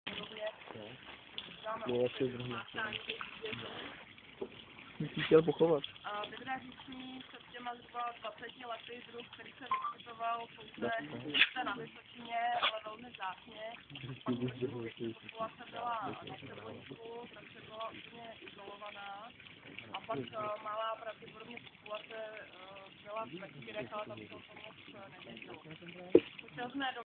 Vydra říční
...jak žere mrtvý kuřata